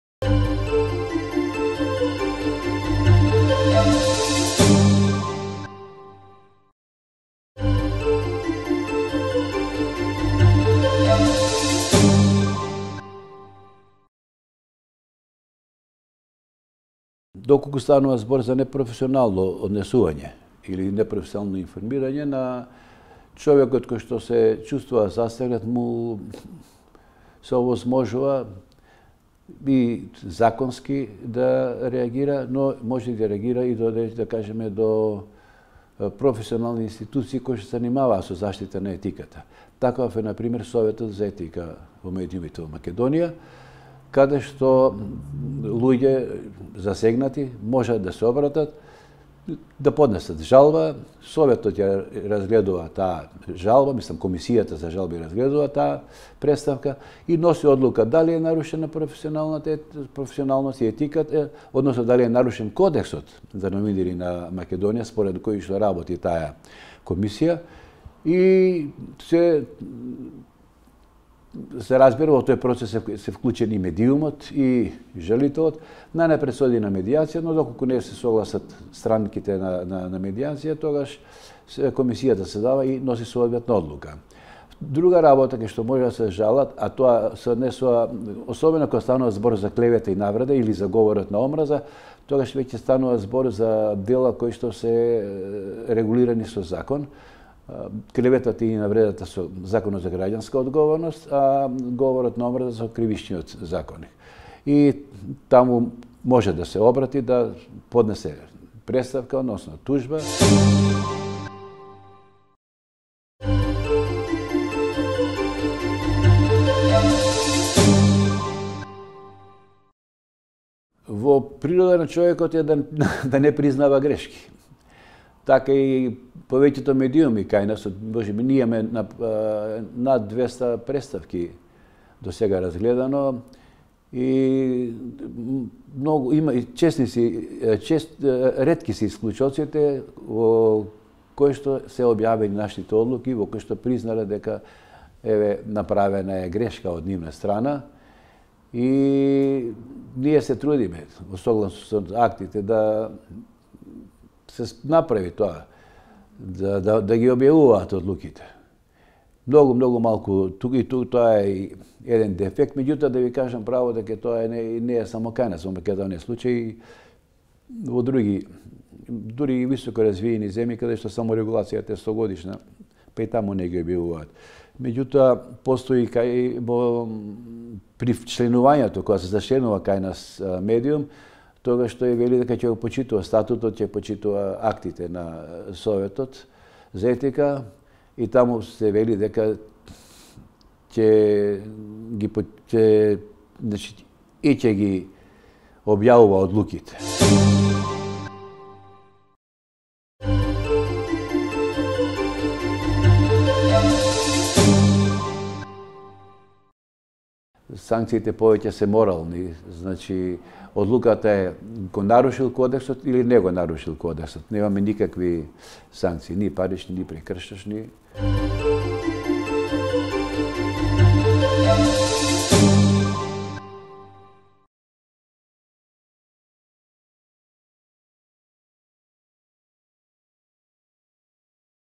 Каде да пријавиме непрофесионално информирање?
Публиката прашува каде и како може да се пожалат граѓаните во случај на непрофесионално информирање? Што ако медиумот не ја признае грешката, и покрај тоа што ја утврдила Комисијата за жалби при СЕММ?  Има ли санкции за медиумот доколку не ја објави одлуката на Комисијата? Одговара Мирче Адамчевски од СЕММ.